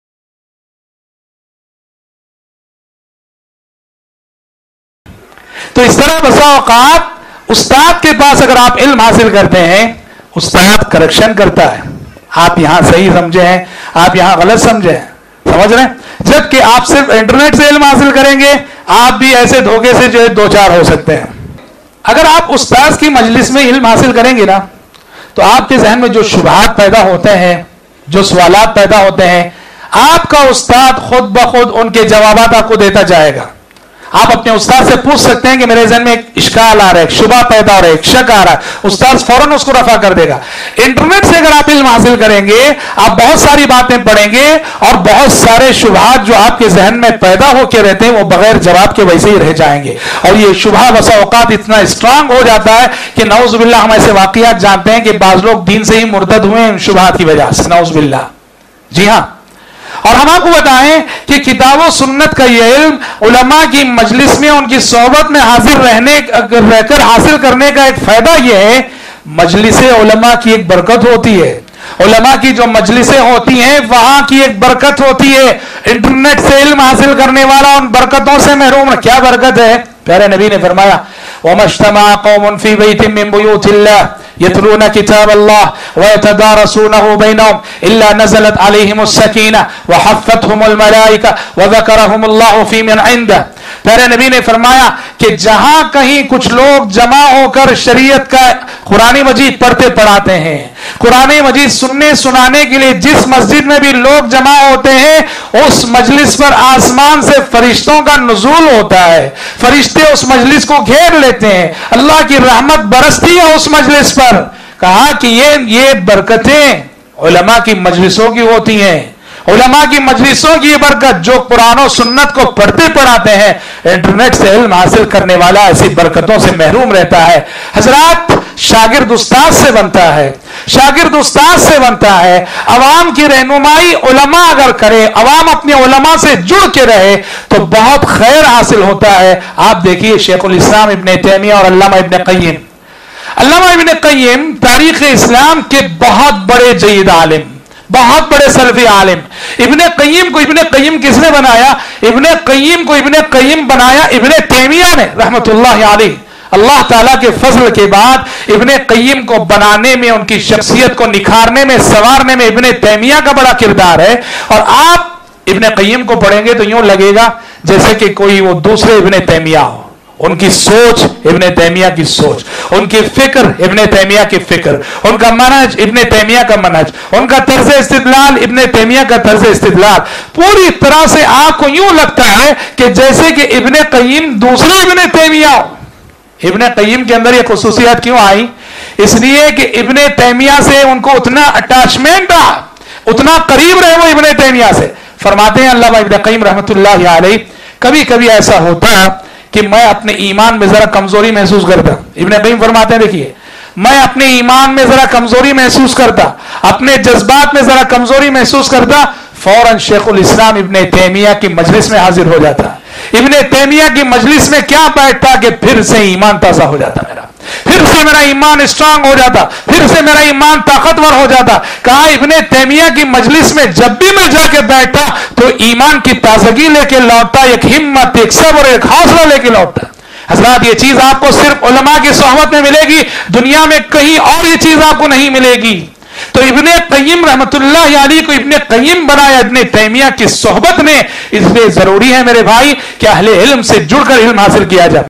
Ulama se ilm Hasil karne ki Barkat | Shagird Ustadh se Banta hai | Ibn Taymiyyah aur Ibn Qayyim
Shaykh Abdul Azeem Umri Madani Hafizahullah

Video Clip Courtesy: IRGC, Chennai Youtube Channel
Full Lecture: Knowledge without the People of Knowledge Shaykh Abdul Azeem Madani